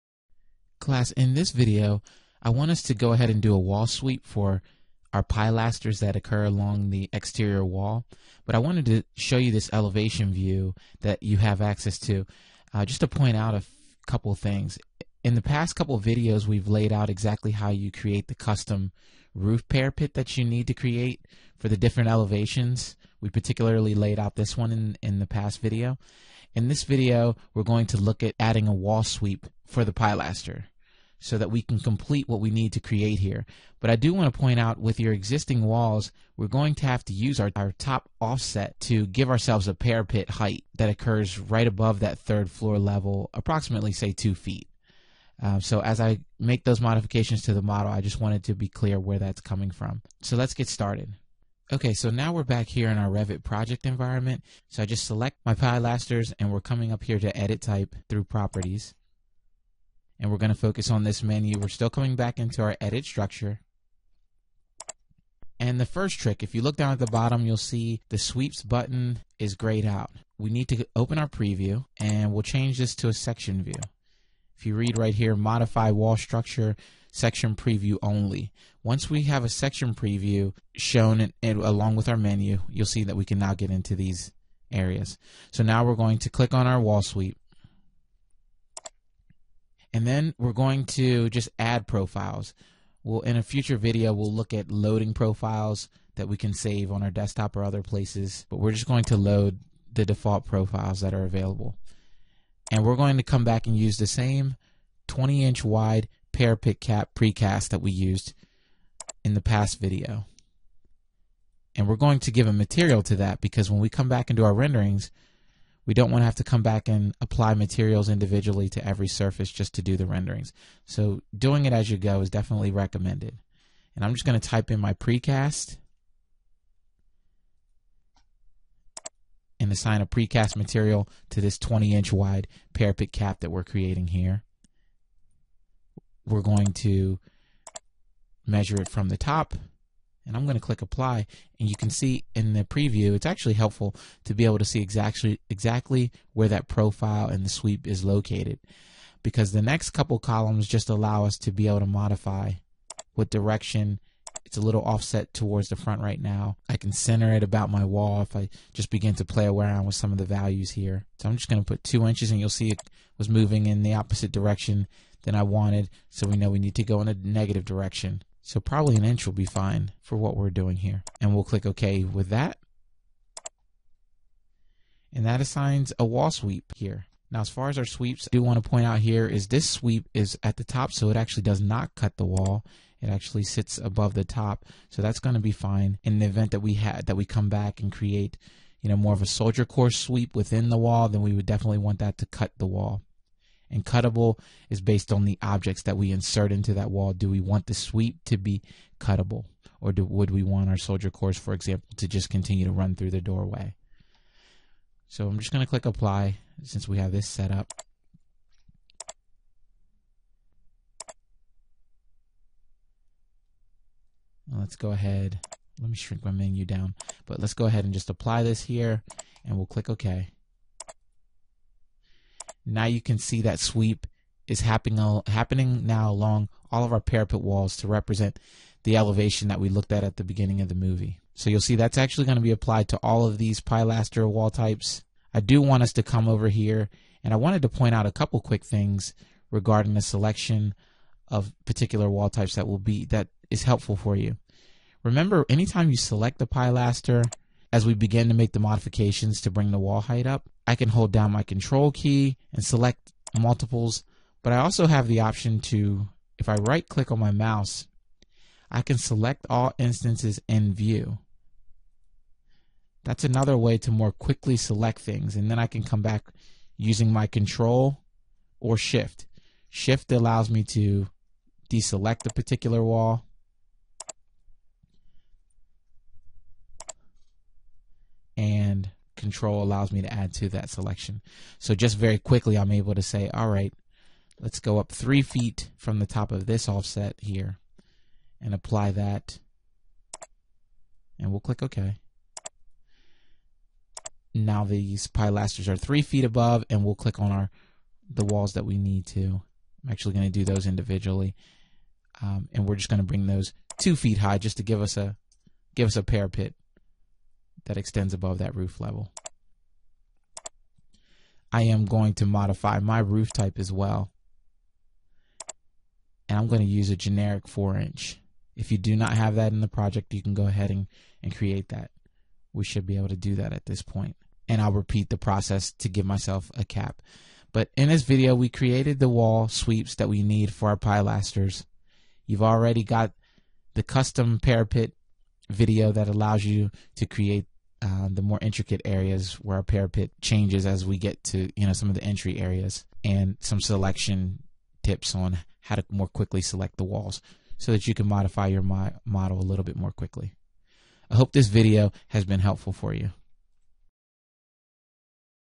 Revit Tutorials: Wall Sweeps
In this Revit tutorial, we will be creating the wall sweep for the pilaster wall in the model.  This wall sweep method will quickly be applied to all walls of a particular type vs individual as done with custom wall sweep created from model-in-place.